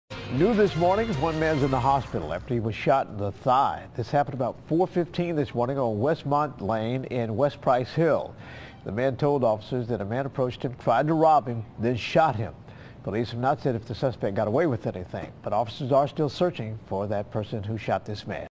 A man shot in the leg in West Price Hill
CINCINNATI (WKRC) -- A man was shot in the thigh early Tuesday morning in West Price Hill.

A Hispanic man said he was shot after another man tried to rob him.

It is not clear if the suspect got away with anything.

The suspect is at large and the victim was taken to a nearby hospital.